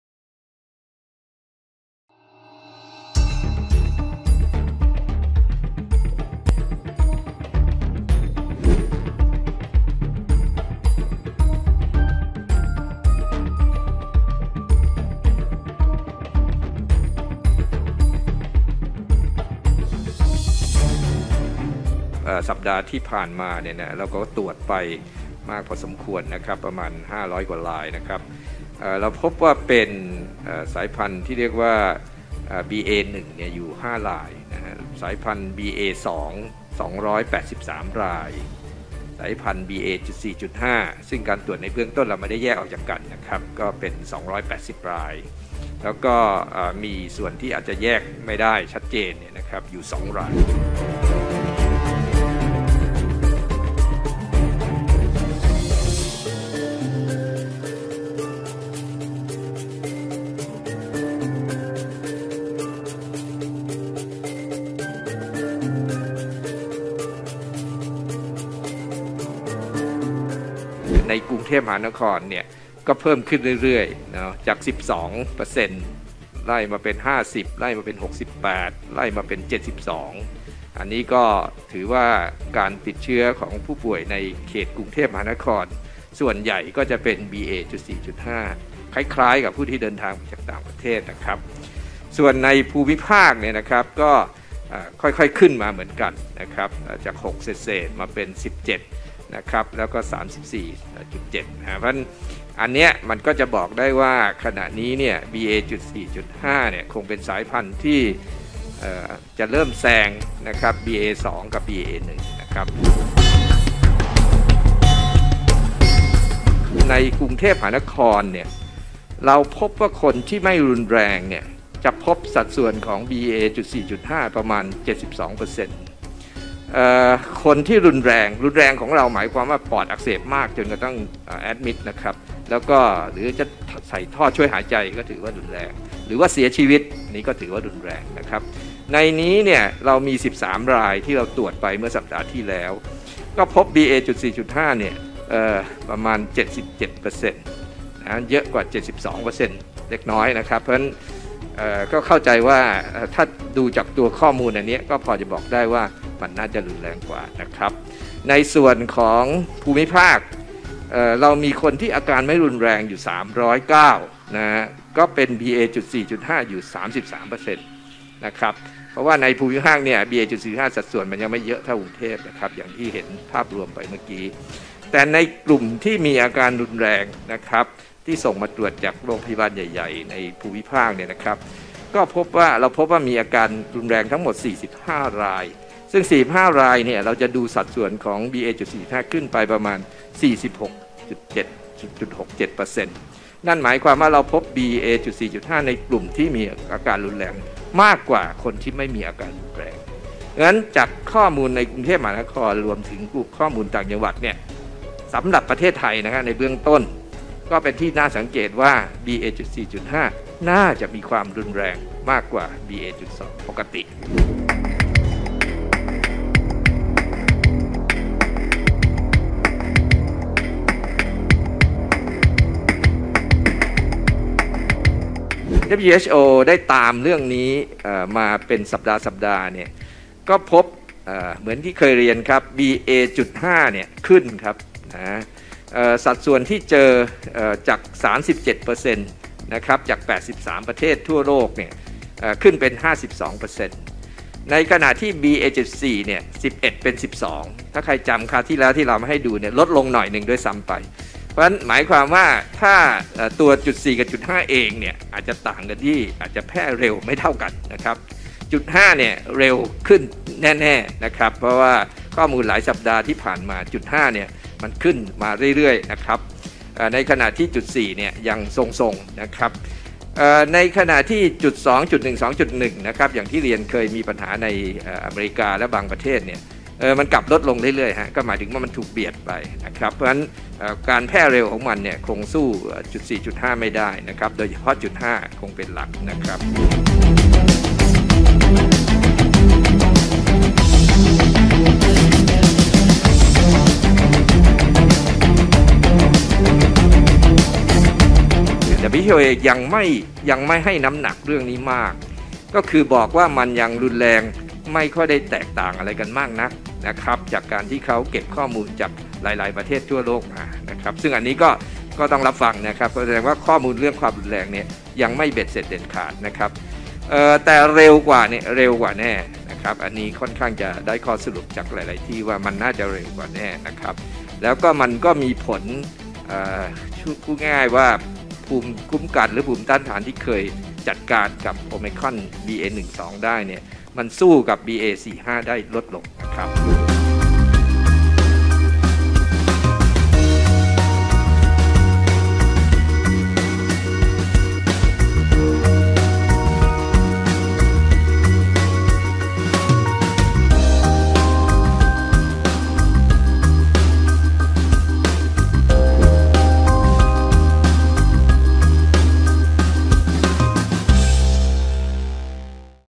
หนักกว่าเก่า BA.4-BA.5 รุนแรงมากกว่าสายพันธุ์อื่น
กรมวิทย์ฯ เผย "โอไมครอน"
BA.4- BA.5 รุนแรงมากกว่าสายพันธุ์อื่น มีความรุนแรงของโรคมากกว่าสายพันธุ์ก่อนหน้านี้ ยืนยันว่า แพร่เชื้อได้เร็วกว่า และสามารถติดเชื้อซ้ำได้